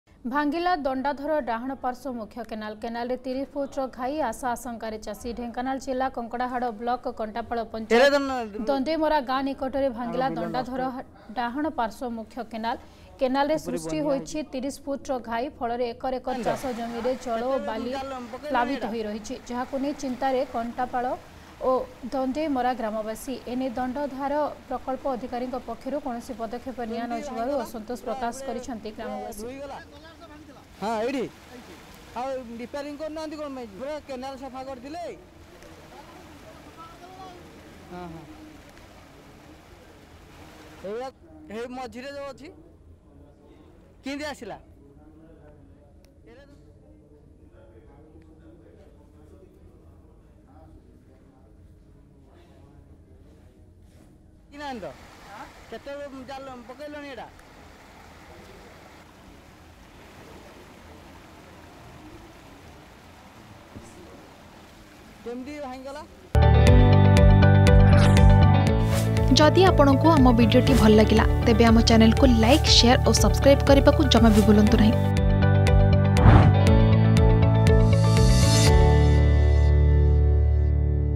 କେନାଲରେ ୩୦ ଫୁଟର ଘାଇ, ଆଶା ଆଶଙ୍କାରେ ଚାଷୀ || Khai Created In canal
ଭାଙ୍ଗିଲା ଦଣ୍ଡାଧର ଡାହାଣ ପାର୍ଶ୍ବ ମୁଖ୍ୟ କେନାଲ। କେନାଲରେ ୩୦ ଫୁଟର ଘାଇ, ଆଶା ଆଶଙ୍କାରେ ଚାଷୀ। ଢେଙ୍କାନାଳ ଜିଲ୍ଲା କଙ୍କଡ଼ାହାଡ଼ ବ୍ଲକ କଣ୍ଟାପାଳ ପଞ୍ଚାୟତ  ଦଣ୍ଡେଇମରା ଗାଁ ନିକଟରେ ଭାଙ୍ଗିଲା ଦଣ୍ଡାଧର ଡାହାଣ ପାର୍ଶ୍ୱ ମୁଖ୍ୟ କେନାଲ।